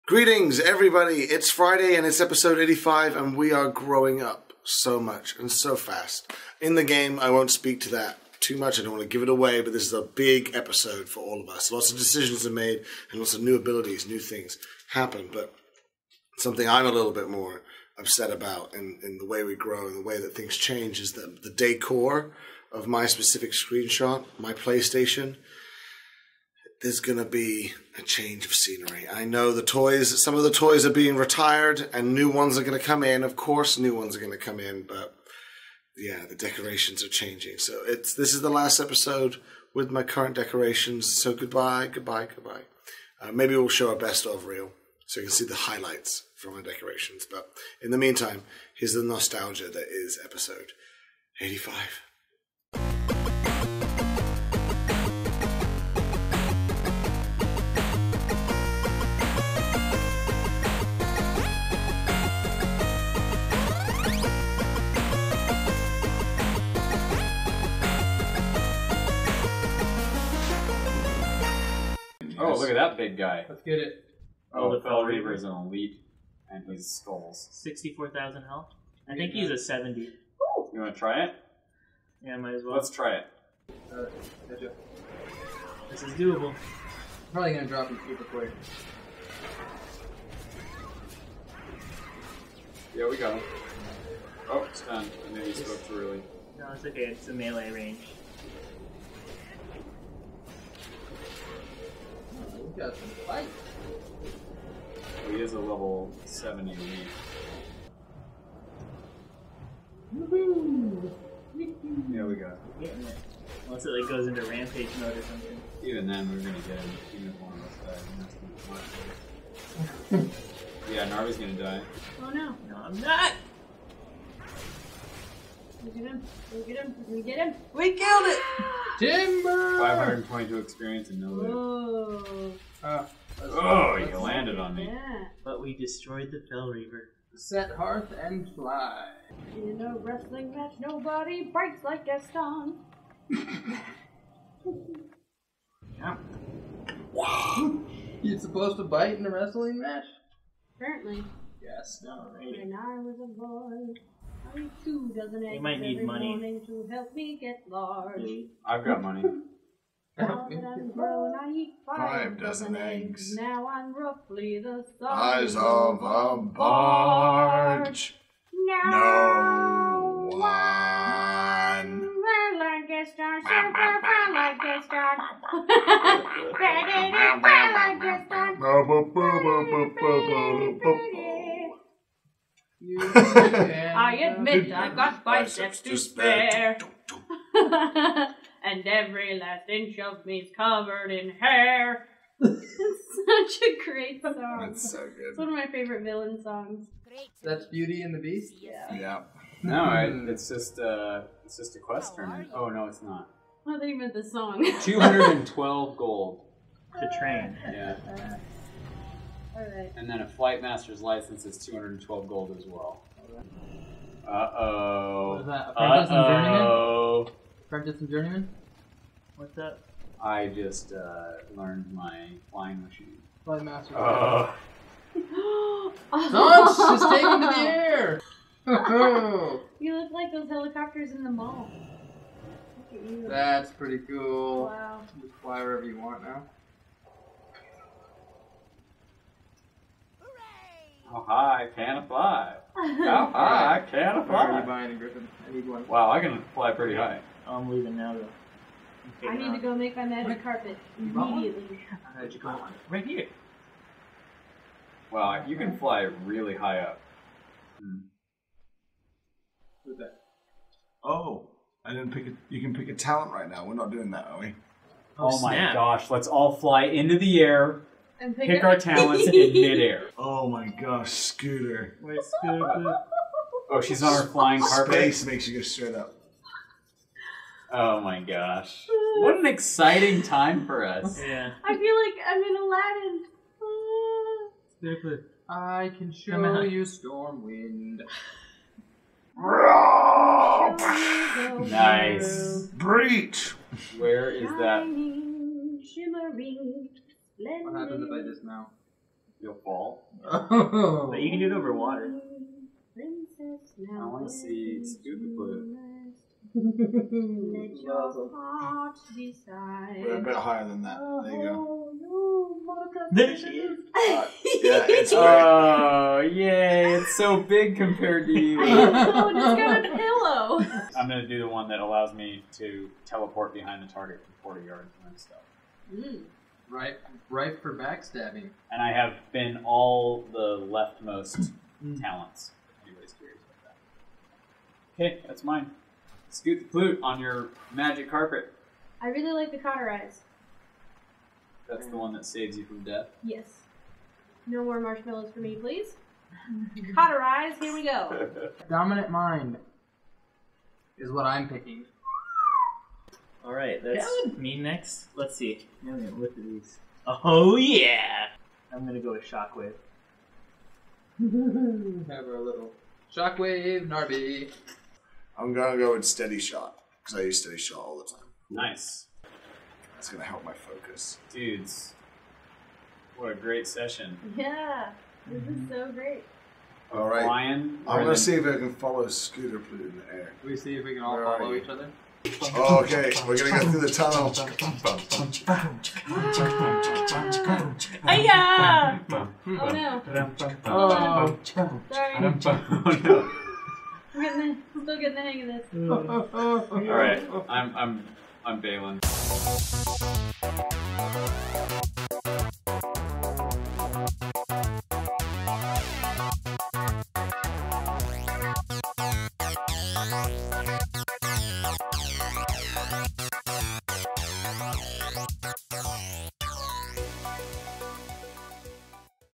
SDBC Ep 85 - Flying Talents
In which Cecilee and James practice for their audition on The Sing-Off, and everyone takes to the skies to learn some new talents.